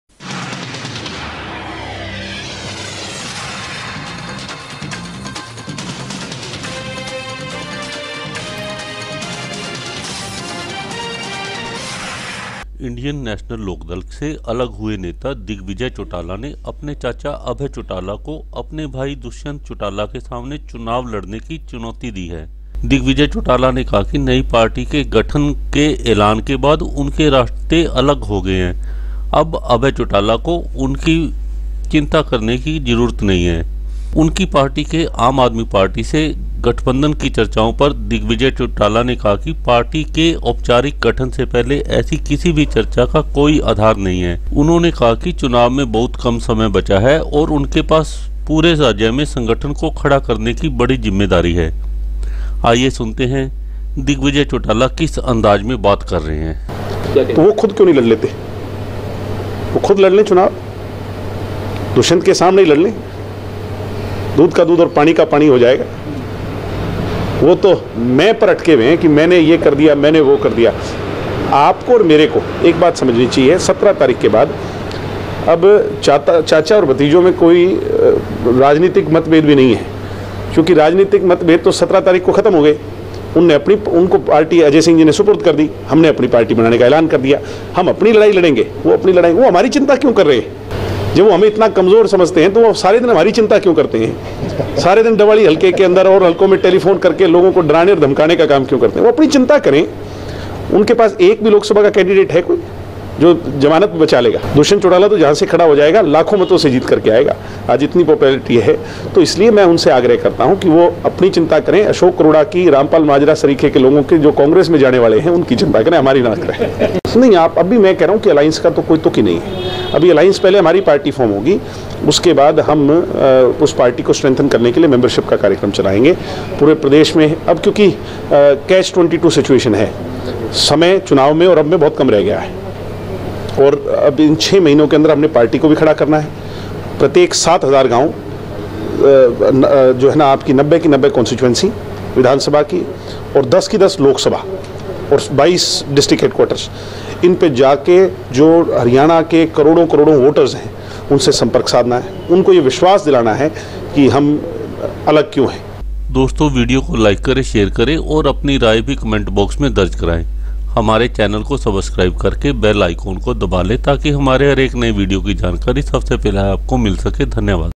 Digvijay Chautala Dares Uncle Abhay Chautala To Fight Election Against Brother Dushyant Chautala
This video is about Digvijay Chautala Dares Uncle Abhay Chautala To Fight Election Against Brother Dushyant Chautala. Digvijay Chautala was reacting to media question about Abhay Chautala comment on new party. Digvijay Chautala said  that after deciding to float new party their family has  no political relation with Abhay Chautala. About alliance with AAP Digvijay Chautala said it is too early to think about any alliance before formal constitution of party.